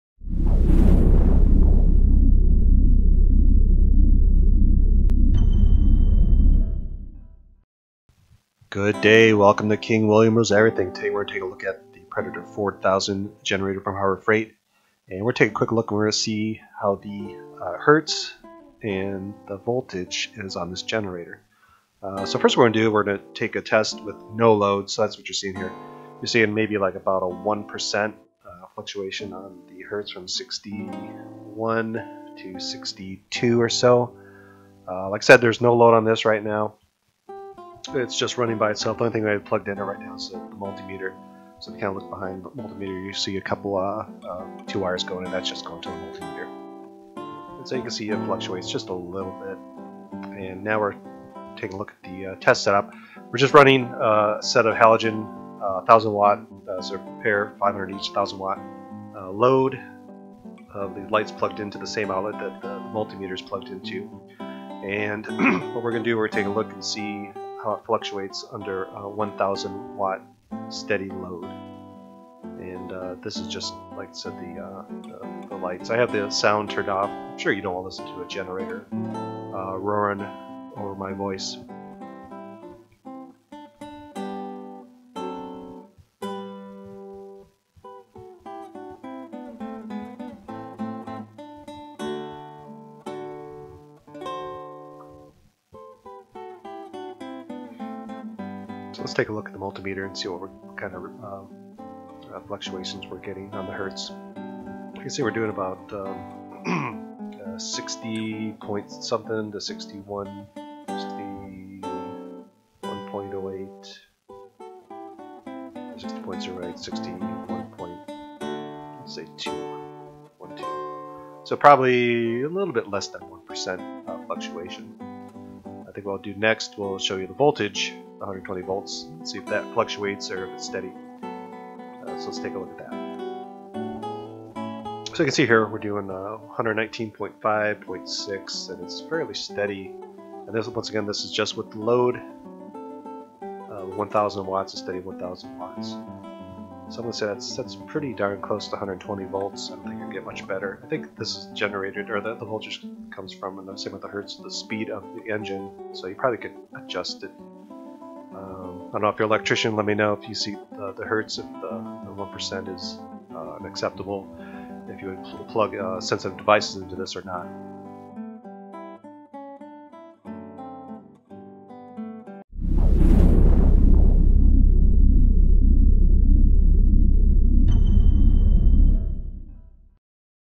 Harbor Freight Predator Generator - Hertz & Voltage Test.
Today we take a look at the Harbor Freight Predator generator to see if the voltage and & hertz has any fluctuations.